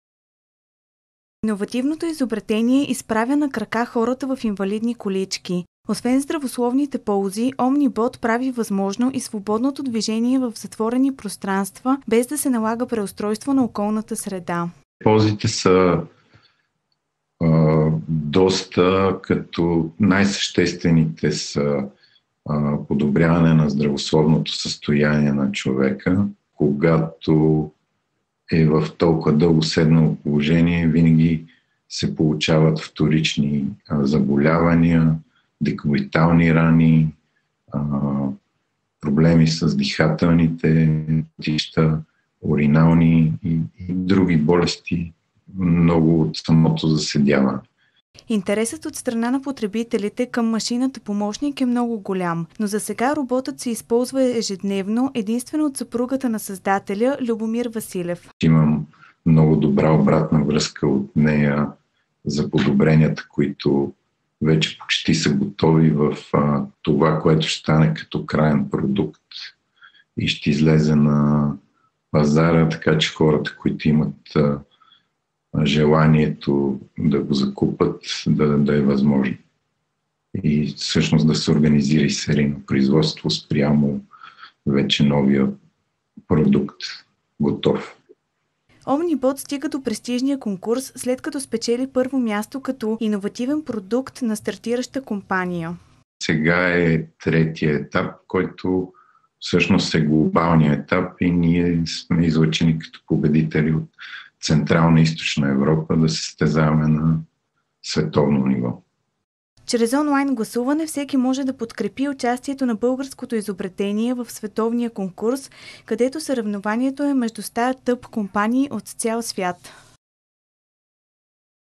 Постижение: Българският Omnibot излиза на световната сцена
Български високотехнологичен робот се бори за световно признание в най-голямото независимо стартъп състезание в света. Гласуването вече е в ход, а победителите ще станат ясни на онлайн събитие на 27 януари.

Иновативното изобретение изправя на крака хората в инвалидни колички. Освен здравословните ползи, Omnibot прави възможно и свободното движение в затворени пространства, без да се налага преустройство на околната среда.